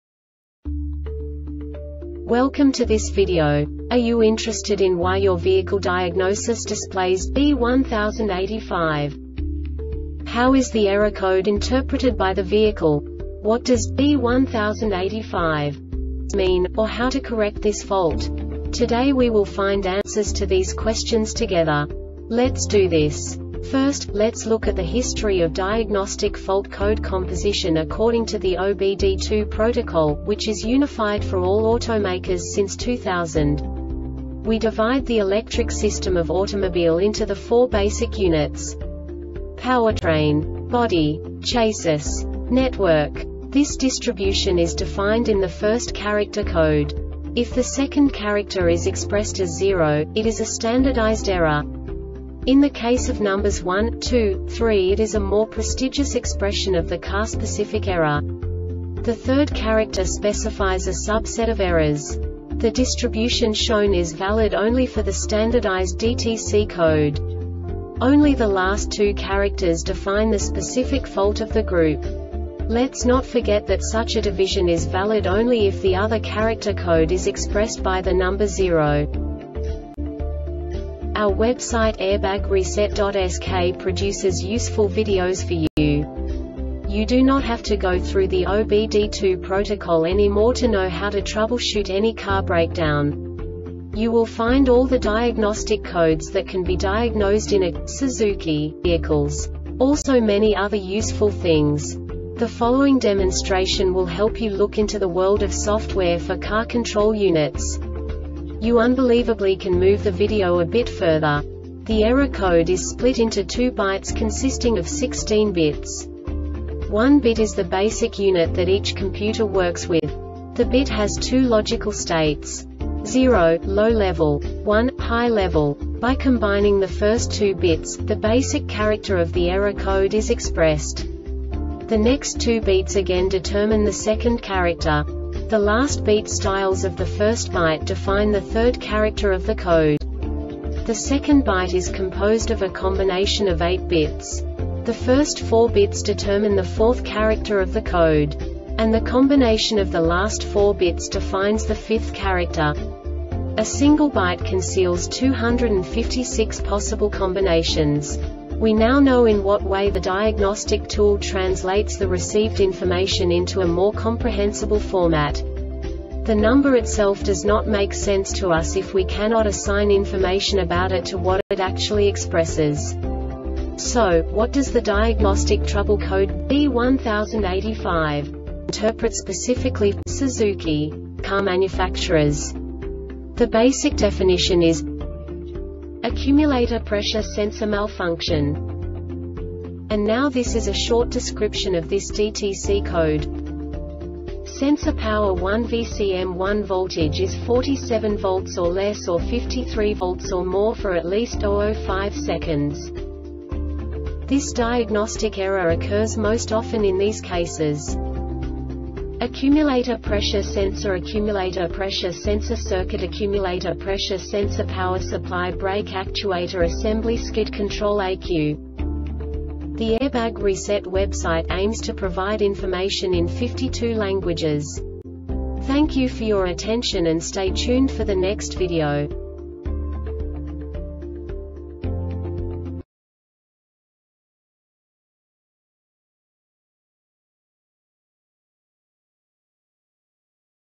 DTC Suzuki B1085 Short Explanation
Contents:
0:21 Basic DTC analysis according to OBD2 protocol standard.
1:48 Insight into programming
2:58 A diagnostically understandable description of the Diagnostic Trouble Code
3:05 Basic error definition

Make:
Suzuki
Code:
B1085
Definition:
Accumulator Pressure Sensor Malfunction
Description:
Sensor power 1 (VCM1) voltage is 4.7 V or less or 5.3 V or more for at least 0.05 seconds.
Cause:
 Accumulator pressure sensor Accumulator pressure sensor circuit Accumulator pressure sensor power supply Brake actuator assembly Skid control ECU